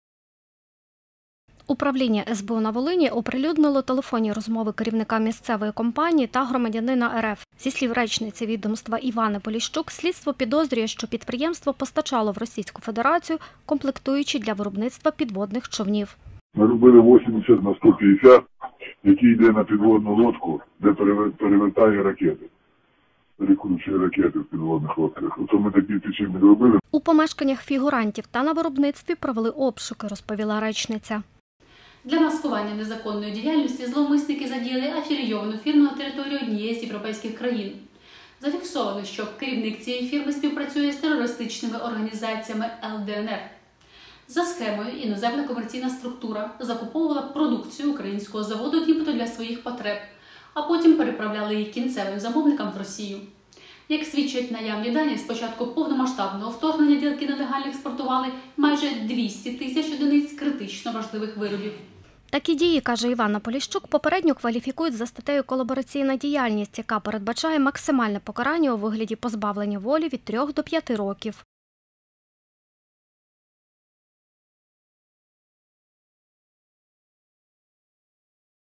Волинська компанія постачала у РФ комплектуючі для підводних човнів – СБУ
На Волині викрили компанію, яка постачала в Російську Федерацію комплектуючі для виробництва підводних човнів, – повідомили в Службі безпеки України області. За матеріалами справи, від початку повномасштабного до РФ нелегально "експортували" майже 200 тисяч одиниць комплектуючих. Назву підприємства правоохоронці не називають.